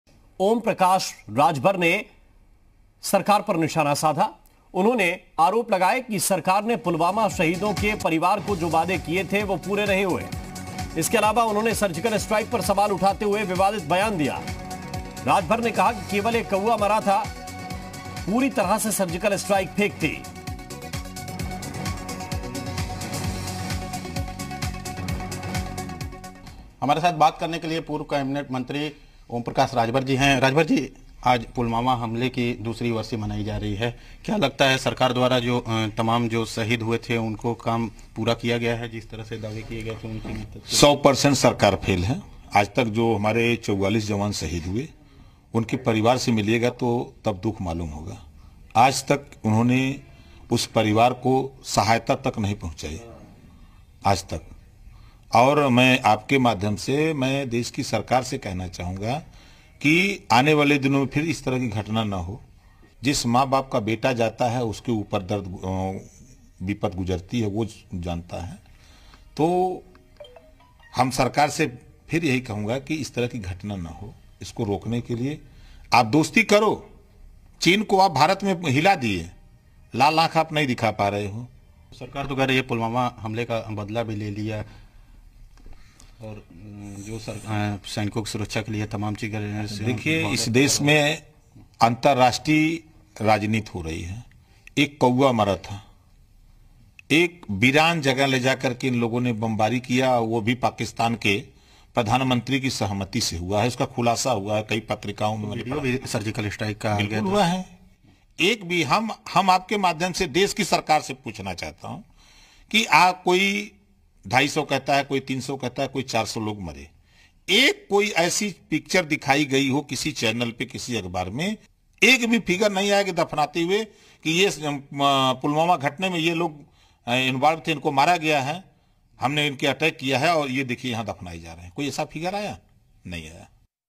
UP News | Om Prakash Rajbhar का Surgical Strike पर बयान Fake था Pakistan पर Attack | Pulwama Hindi
ओमप्रकाश राजभर (Om Prakash rajbhar) ने सरकार पर निशाना साधा है।  उन्होंने आरोप लगाए कि सरकार ने पुलवामा शहीदों  (pulwama martyrs )के परिवार को जो वादे किए थे वो पूरे नहीं हुए। इसके अलावा उन्होंने सर्जिकल स्ट्राइक  (Surgical Strike )पर सवाल उठाते हुए विवादित बयान दिया है।  राजभर ने कहा कि केवल एक कौवा मरा था पूरी तरह से सर्जिकल स्ट्राइक फेक थी।